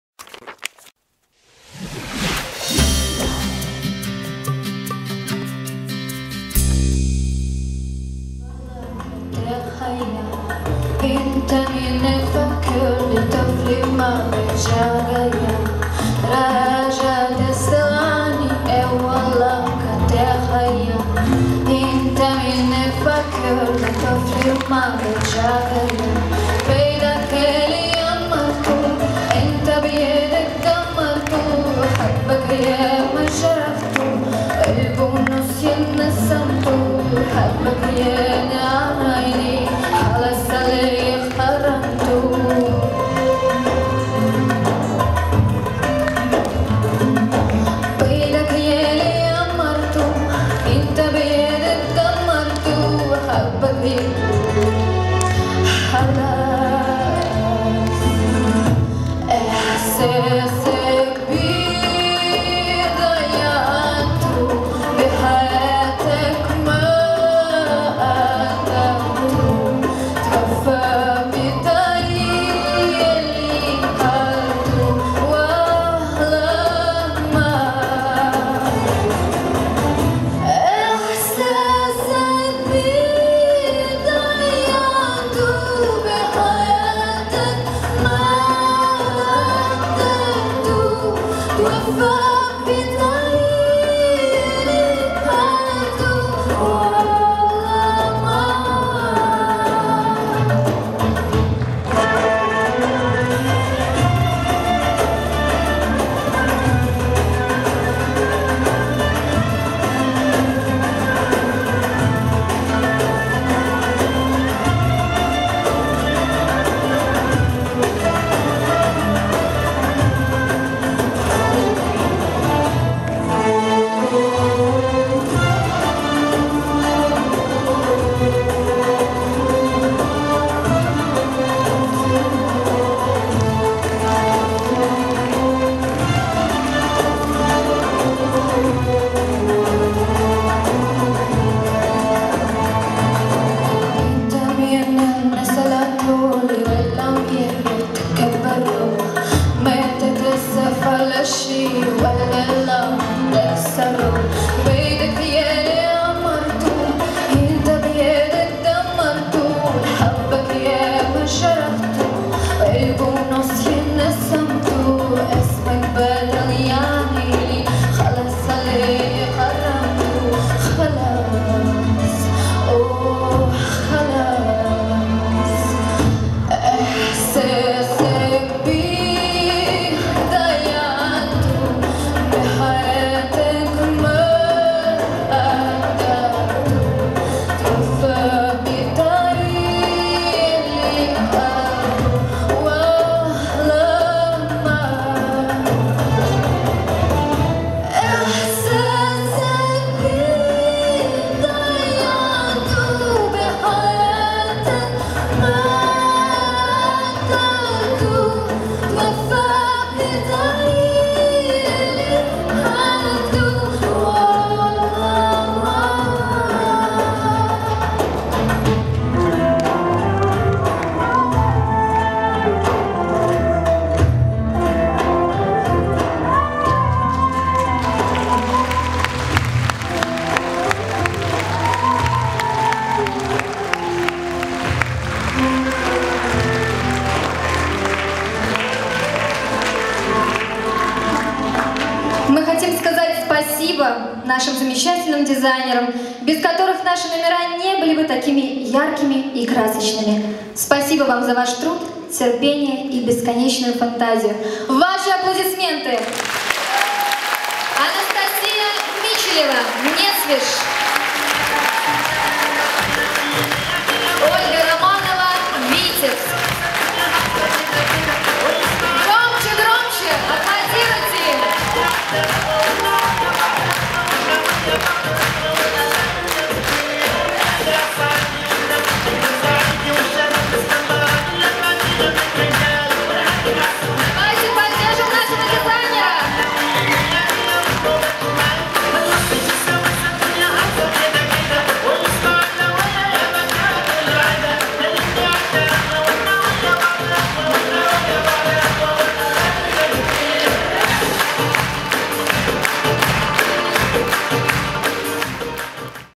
Дизайнерский показ костюмов от Анастасии Мичелевой и Ольги Романовой. Поет Анна Храпунович.
Отчетный концерт профессиональной школы восточного танца и трайбла Azalais в городе Витебск.

Организаторы Анастасия Постнова и Ани Авагимян.